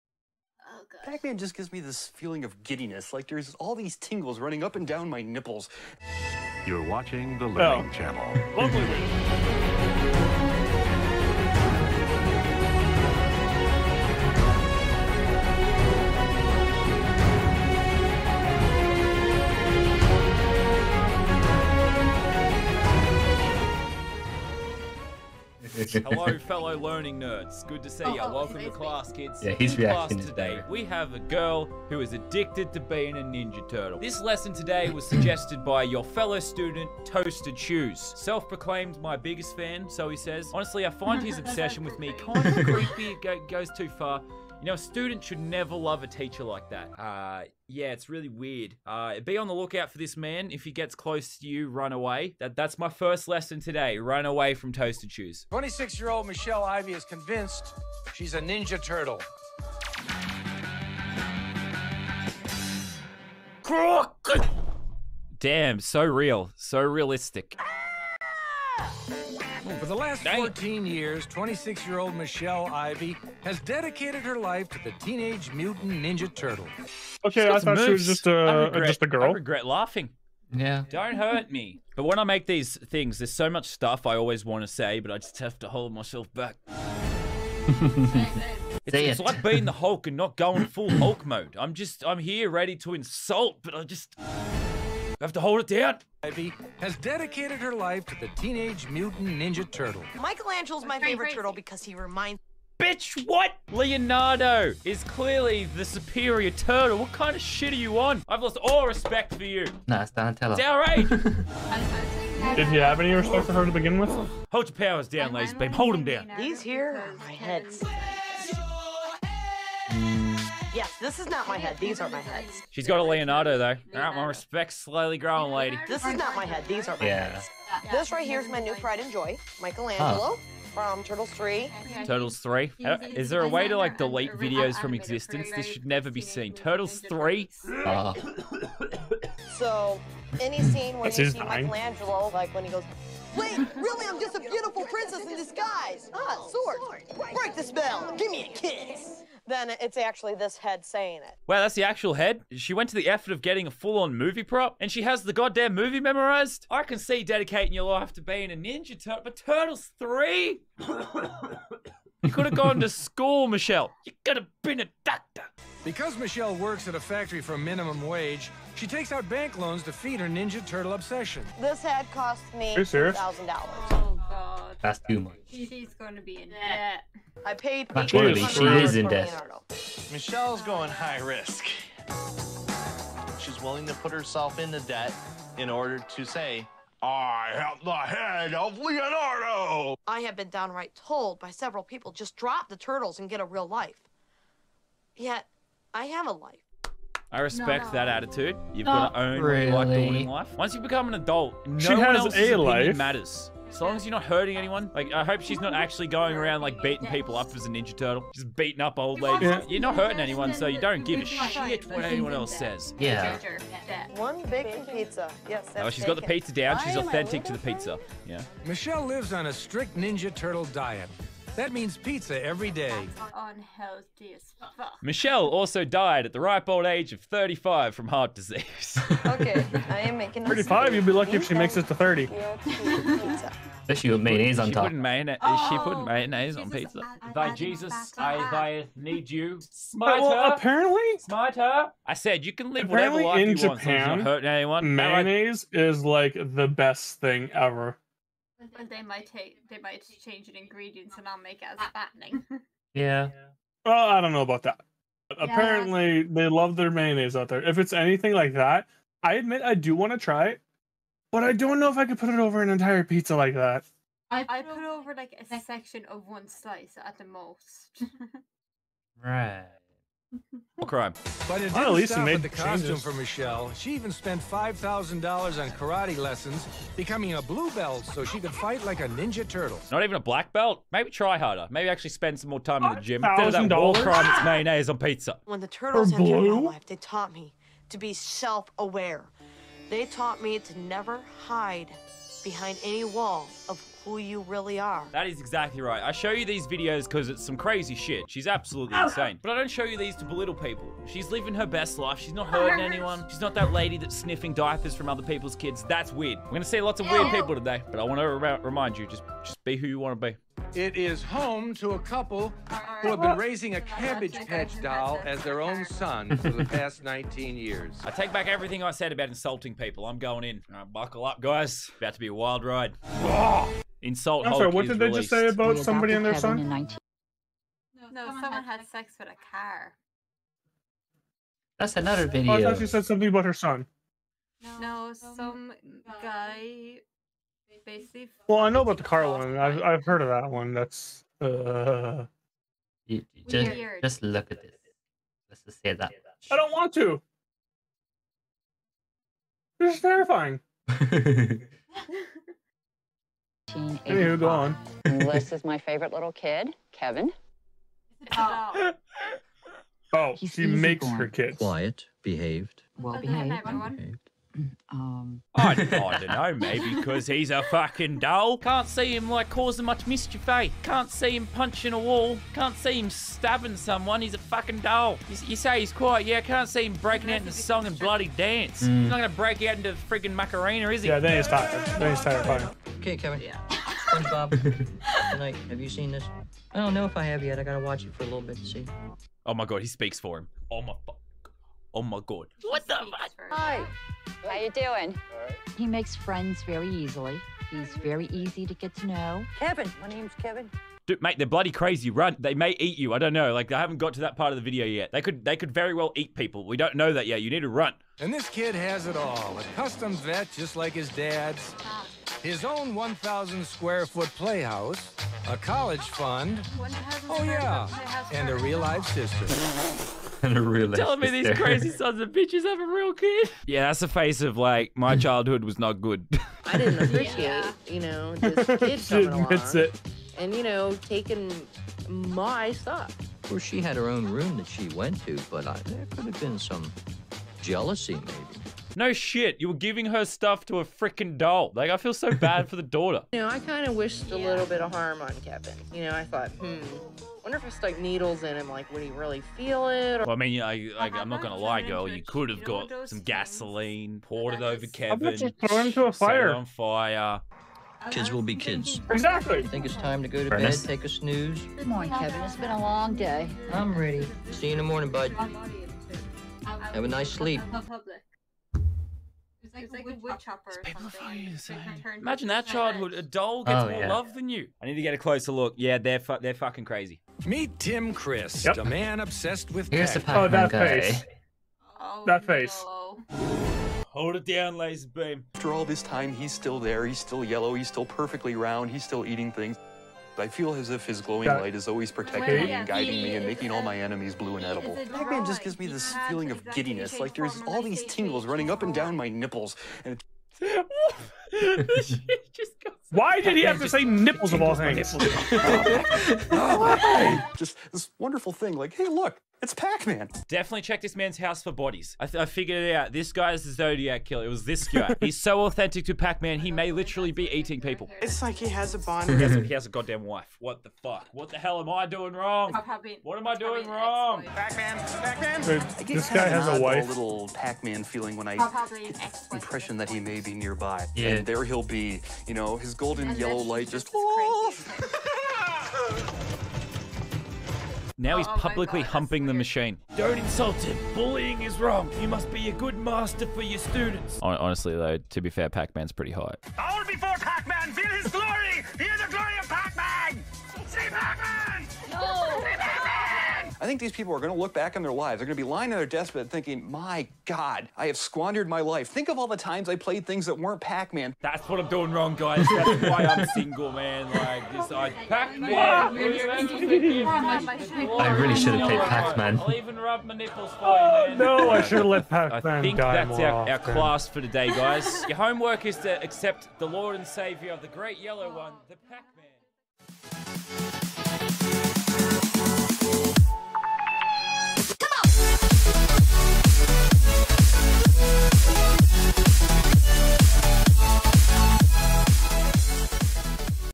Creepy Guy loves PacMan *Questionable*
Reacting to Pacman lover  ft @aegis7251 & @glicfoxart